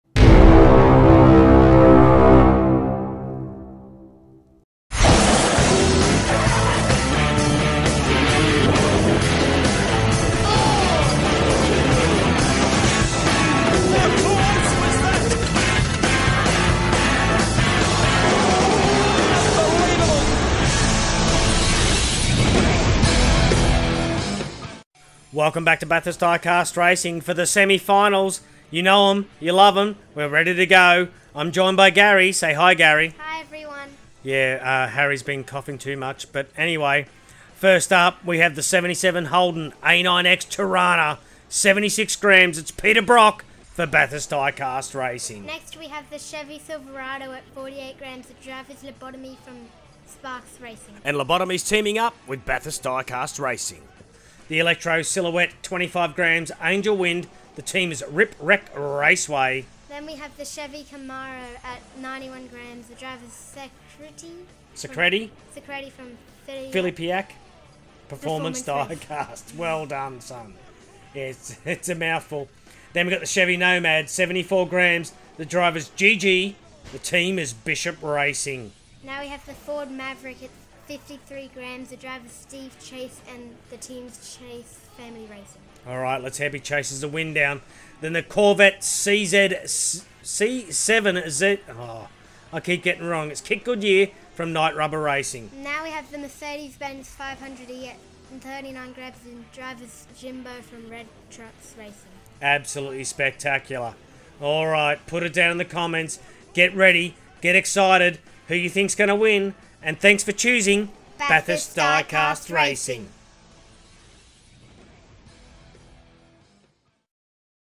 READY FOR SOME REAL ACTION? Semi Finals 31&32 You Choose
THE INTERNATIONALS ARE BACK!!!

This is going to be EPIC!

This hand carved, outdoor Bathurst Track Replica races 1/64 scale Hotwheels, Matchbox and Binate cars against each other to find out who will be the champion of Mount Panorama Bathurst.
We can also control the weather!
Let's find the next King of the Mountain!
Round 3 Semi Finals - You Choose 31&32